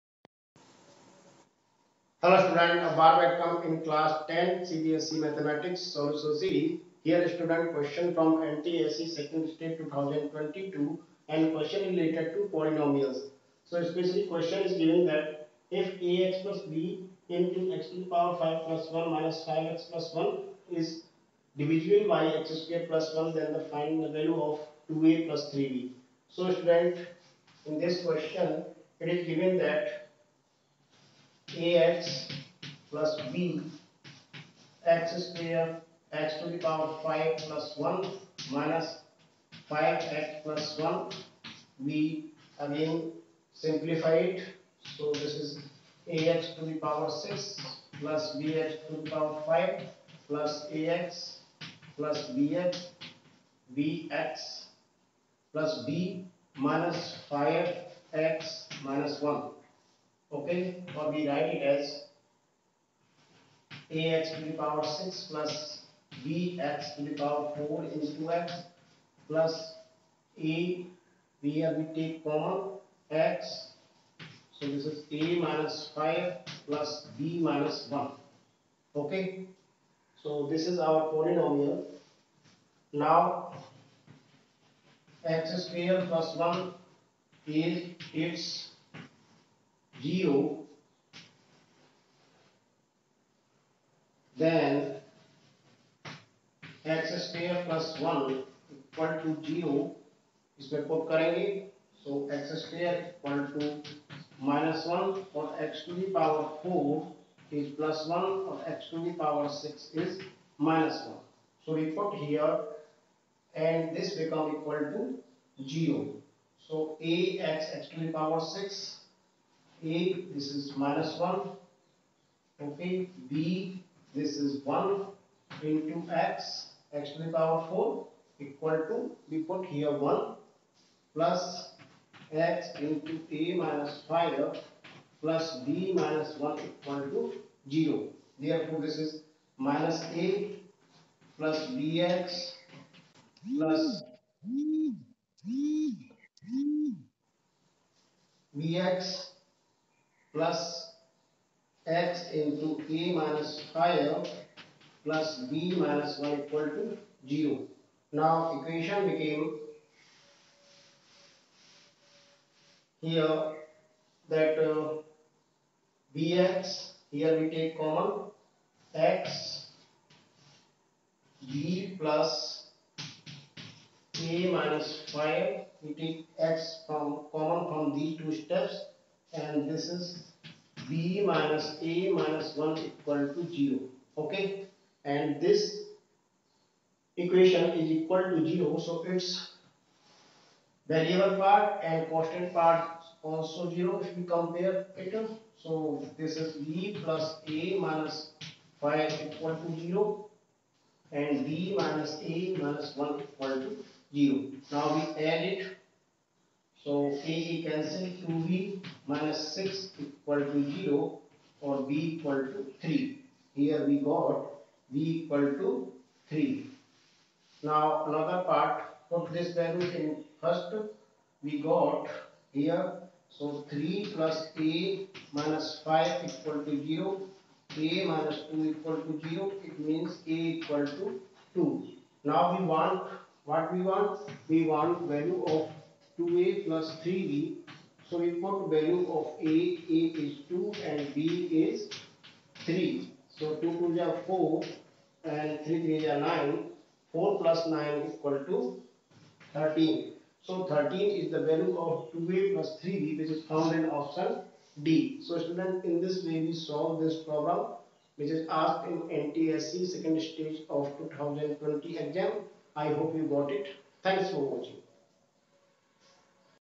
If (ax+b)(x⁵+1)-(5x+1) is divisible by x²+1 then the value of 2a+3b is | ntse Maths stage 2 ,2020
If (ax+b)(x⁵+1)- (5x+1) is divisible by (x²+1 ) then find 2a+3b
ntse maths stage 2 2020
Class 10 CBSE Maths
CBSE Class 10 Maths Solution by Kcp Sir